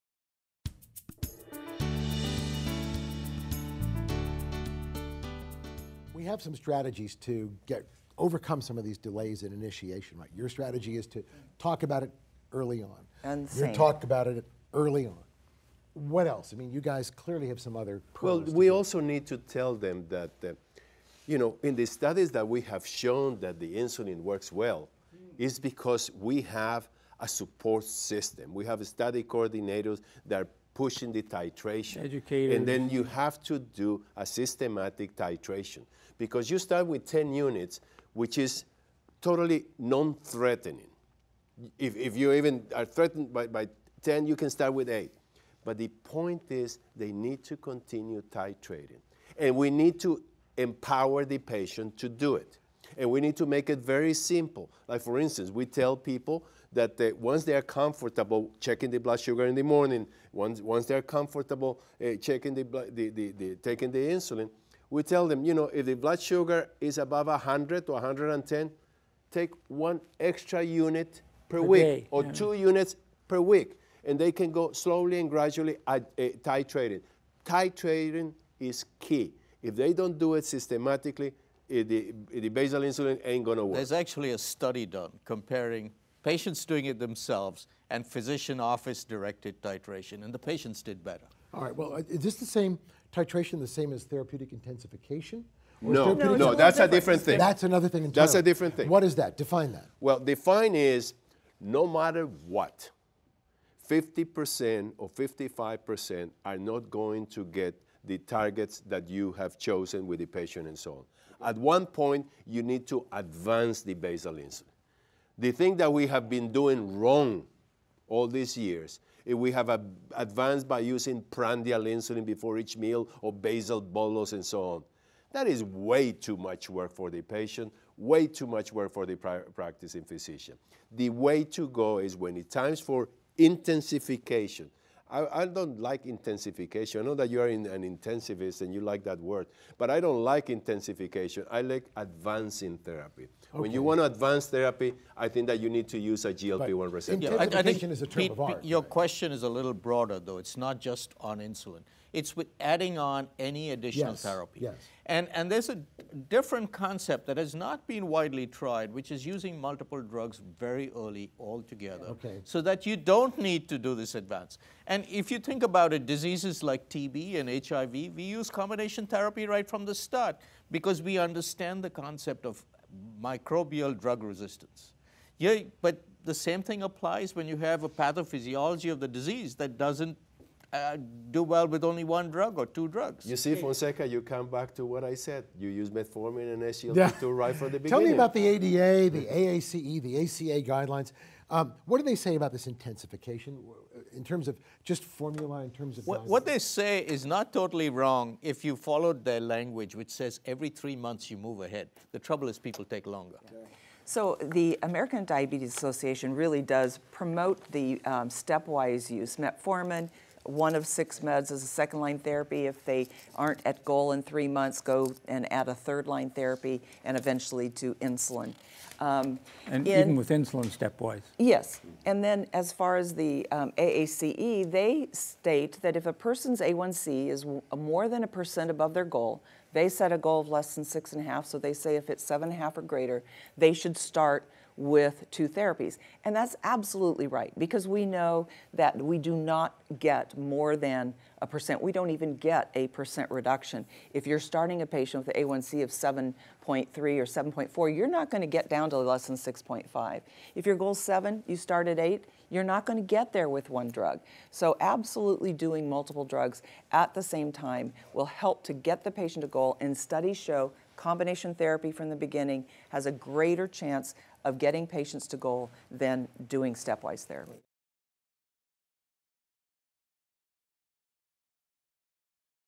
Early Combination Therapy in Type 2 Diabetes
In this segment, Vivian Fonseca, MD; Robert Henry, MD; Julio Rosenstock, MD; Peter Salgo, MD; and Carol Wysham, MD, discuss the optimal way to titrate insulin therapy as well as the rationale for an intensive upfront approach in type 2 diabetes.